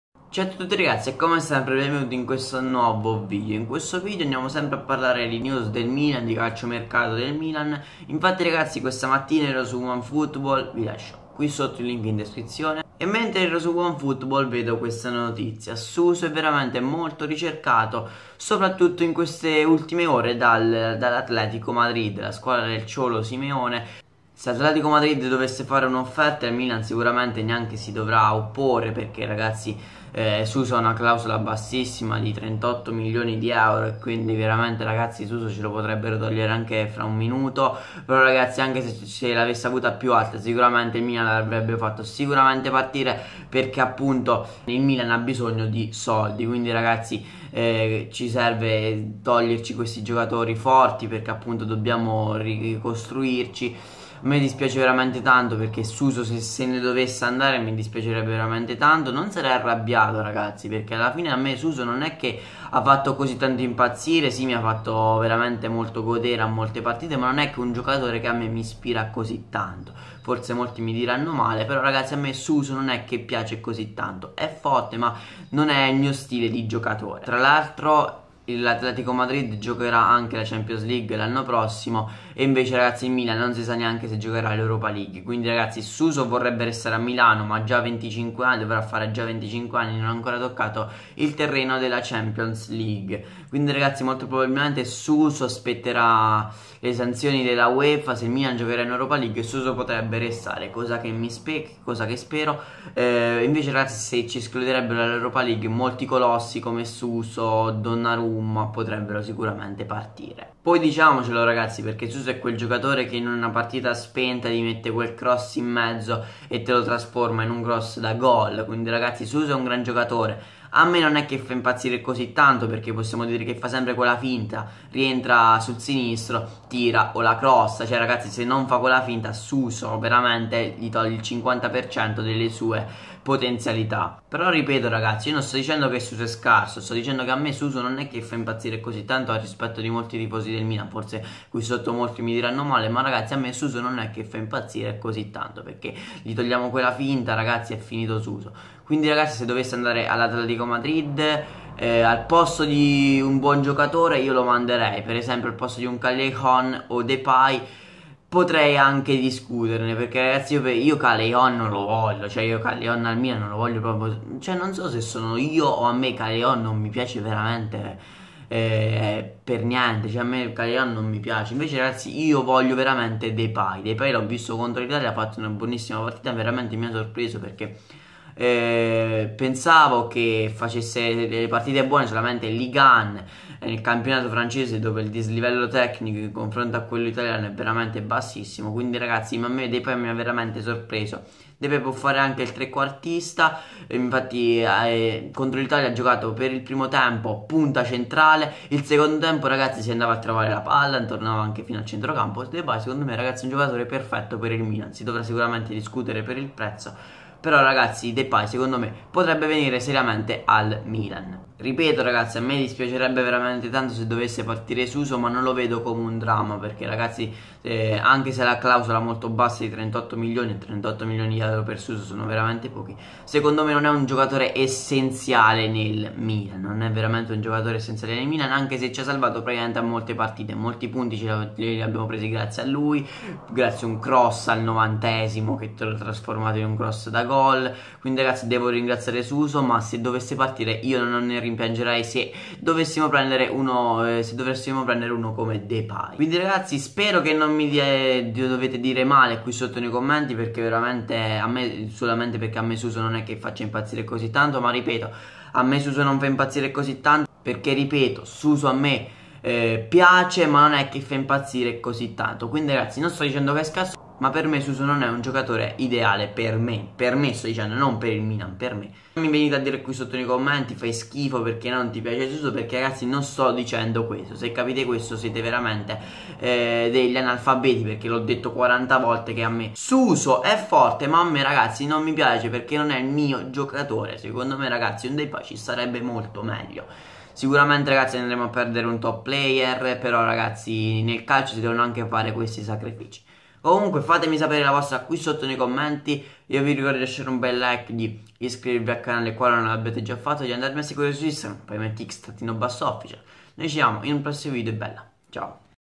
L'ATLETICO PAGA la CLAUSOLA di SUSO!?É il MOMENTO di fare A MENO di LUI??[Calciomercato Milan]
Sembrerebbe che l'Atletico Madrid sia pronto a pagare la clausola di suso...ne parlo in questo nuovo video